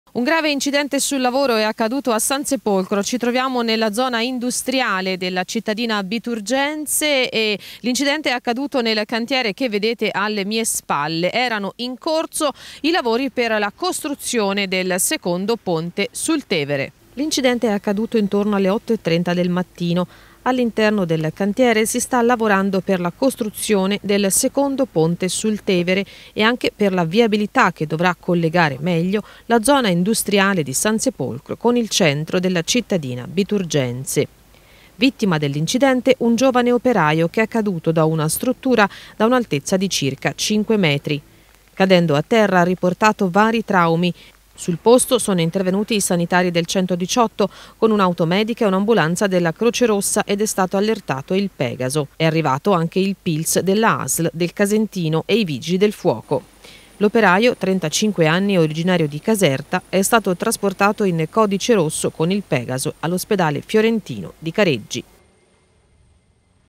Incidente sul lavoro: operaio fa un volo di 5 metri
Ennesimo incidente sul lavoro in provincia di Arezzo. È successo questa mattina intorno alle 8:30 nella zona industriale di Sansepolcro, Valtiberina aretina. L’incidente si è verificato in un cantiere per la costruzione di un ponte sul Tevere. Un giovane operaio è caduto da una struttura da un’altezza di 5 metri. Cadendo a terra ha riportato vari traumi. Sul posto  sono intervenuti  Pegaso 1, un’auto medica , un’ambulanza della Croce Rossa, il PISL della Asl del Casentino ed i Vigili del fuoco. L’operaio, 35enne originario di Caserta, è stato trasportato in codice rosso all’ospedale di Careggi.